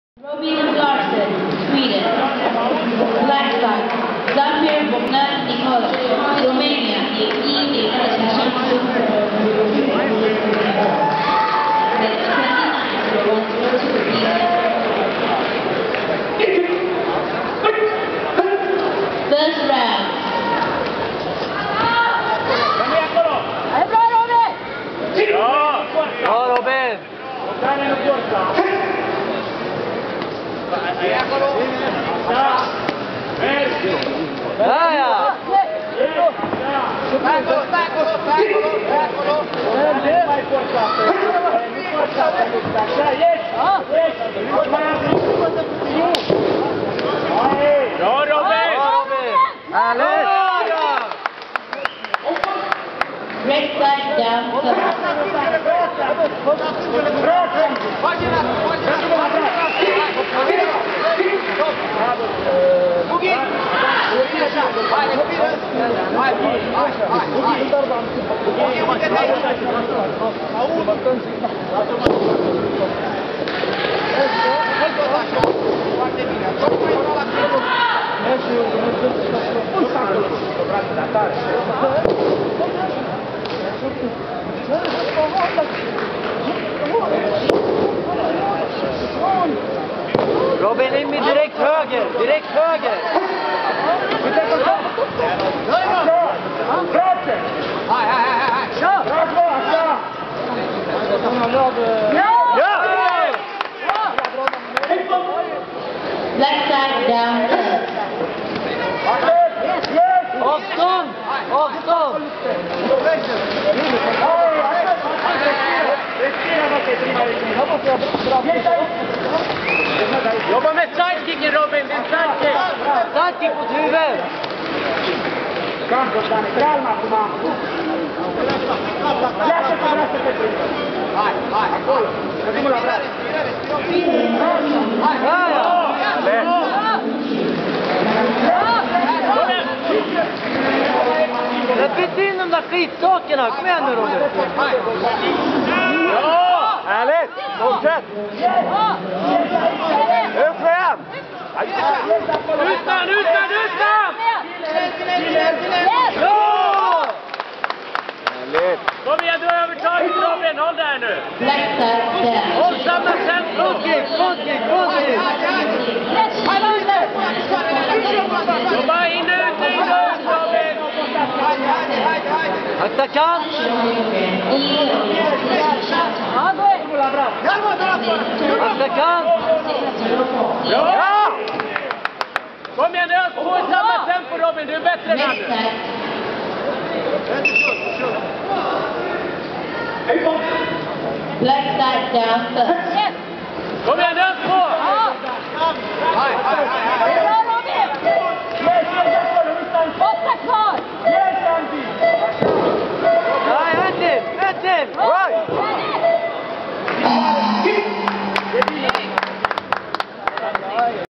Robin Larsson SWE vs Romania 12th WWC 2013 sanshou, 1 round
Robin Larsson SWE vs Romania 12th WWC 2013 sanshou, 1 round, Romania winner